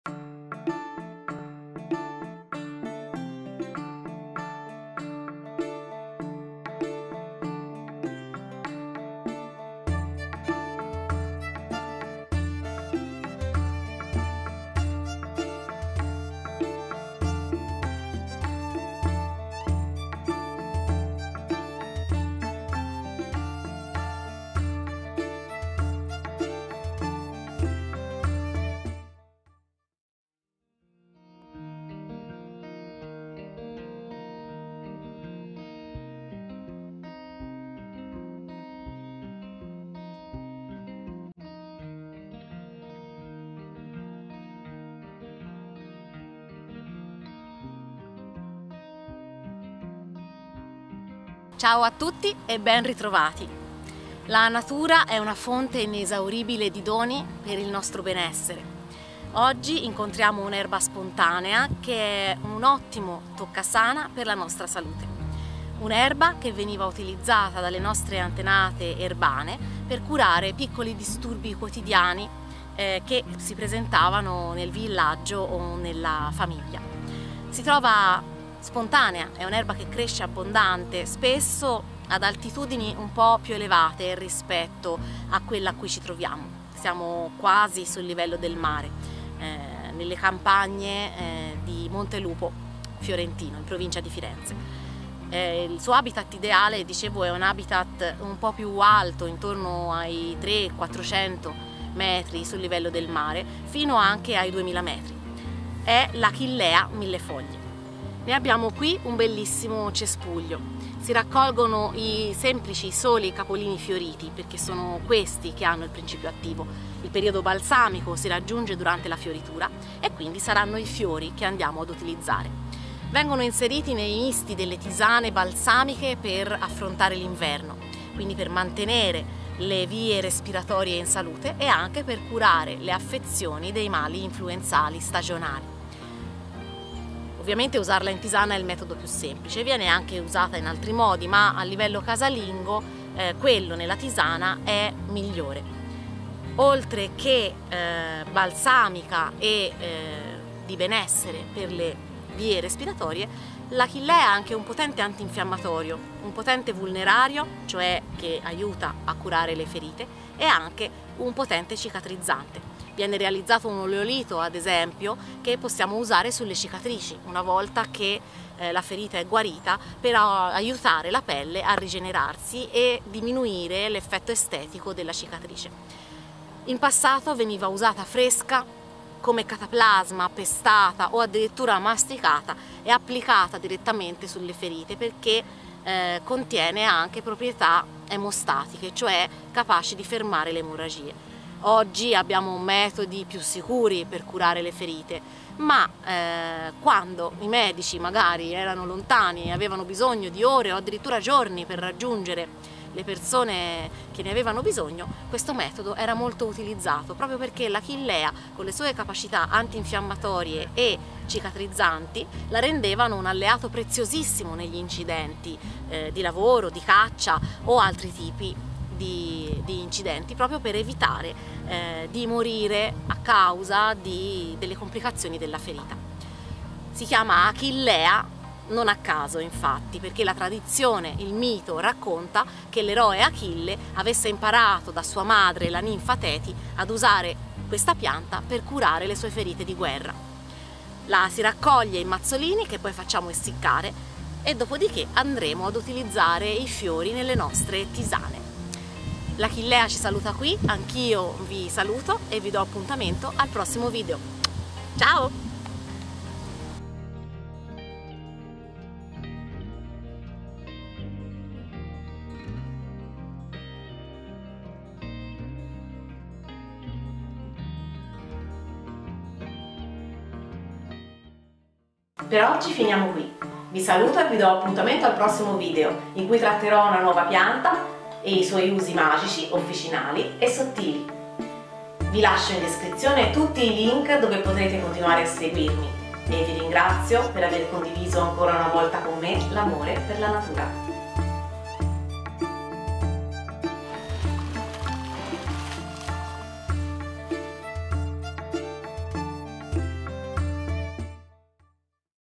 Fabiola e le erbe officinali e magiche: l'achillea millefoglie
Andiamo a conoscere una delle signore dei prati dalle mille virtù!

🌸Se ti è piaciuto il video continua a seguirmi su Youtube e anche su Facebook:

Video realizzato e montato da Lisa: @SpiritoDellaNatura